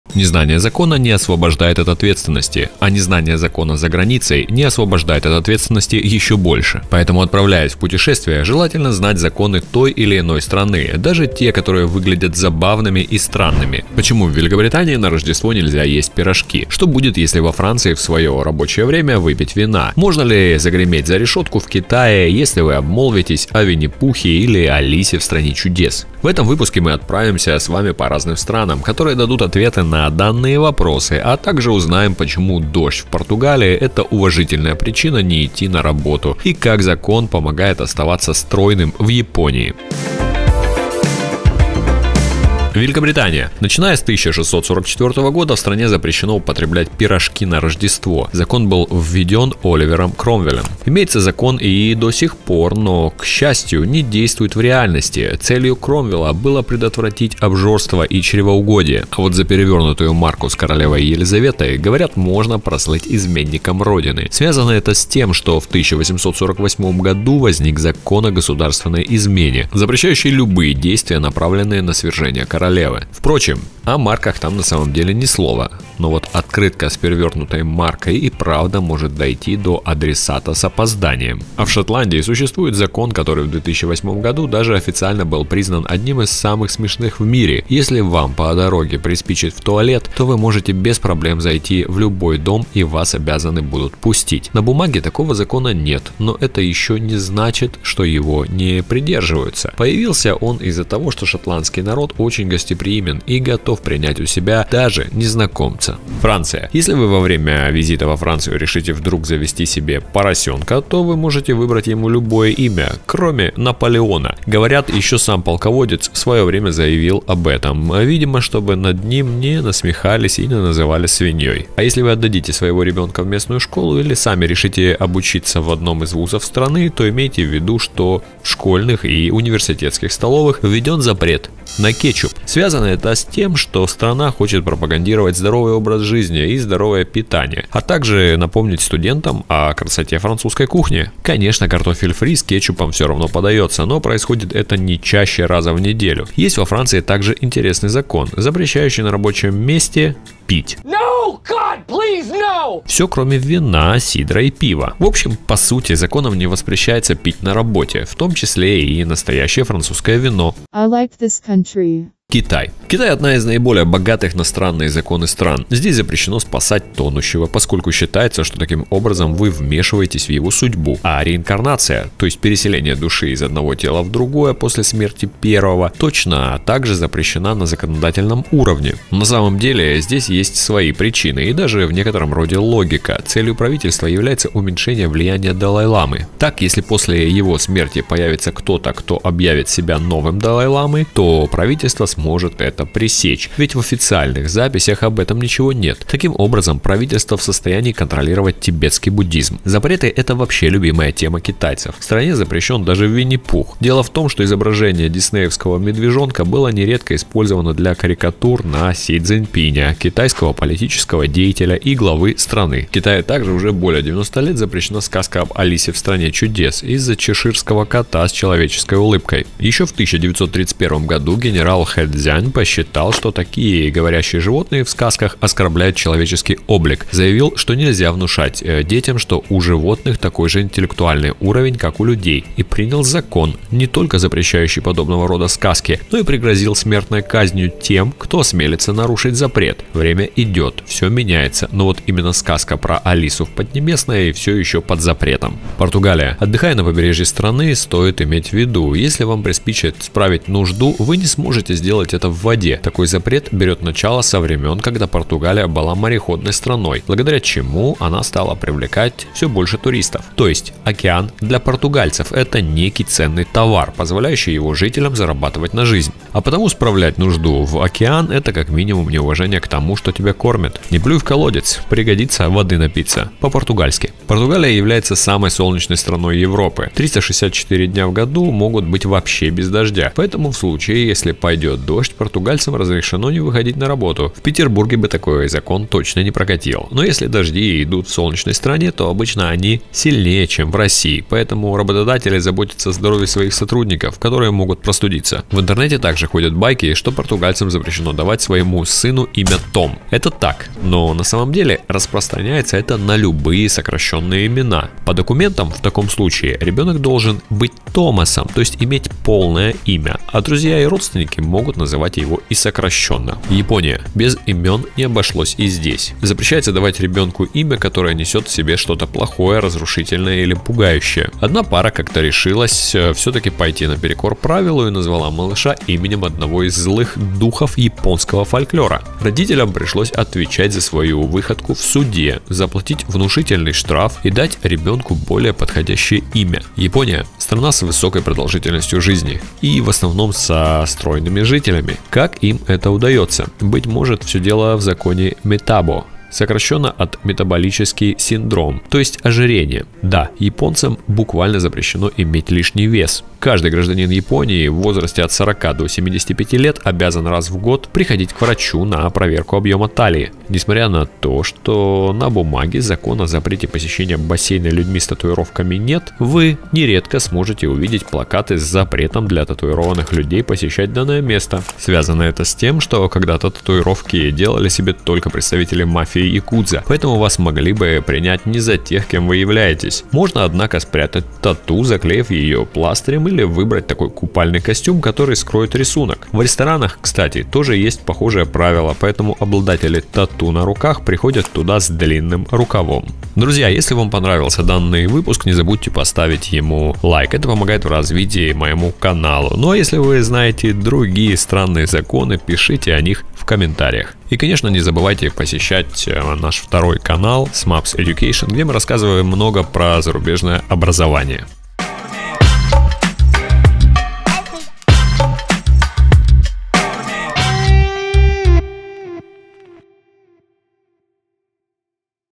Почему нельзя спасать тонущего и где запрещено толстеть? Самые странные законы в мире
Самые странные законы в мире: почему нельзя спасать тонущего и где запрещено толстеть?

Всем привет, с вами Егор Еремеев и этой мой канал о путешествиях! При посещении той или иной страны важно знать о её законах, ведь некоторые из них для нас удивительны. Многие из них были сформированы в течении истории страны, религии или из-за уникального климата. Но за нарушение может последовать наказание в размере больших штрафов. Поэтому перед тем, как ехать в страну – изучите её особенности!

А про некоторые я расскажу в своем видео:

1:43 Закон о пирожках
1:22 Как законно попасть в любой дом
1:46 Какие имена запрещены животных
2:26 Алкоголь во время рабочего дня
2:43 Можно ли спасать тонущего?
3:20 Запрет на Винни-Пуха и Алису из страны чудес
4:05 Ценность океана
4:34 Можно ли остаться дома из-за дождя?
5:14 Запрещенные имена для детей
5:40 Запрет на лишний вес